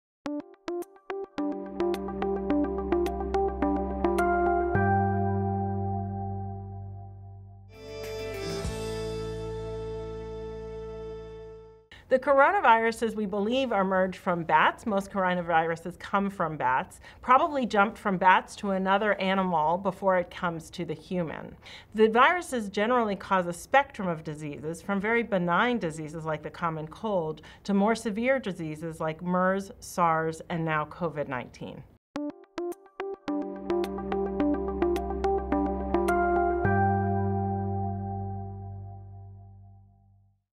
Coronavirus Q&A: Mass General Physician Explains COVID19 and its History
What is coronavirus and where did it originate from? In our coronavirus Q&A series, Mass General physicians answer common questions about COVID-19 in these short clips. Here, Rochelle Walensky, MD, MPH, chief of infectious disease at Mass General, explains the nature and history of the virus.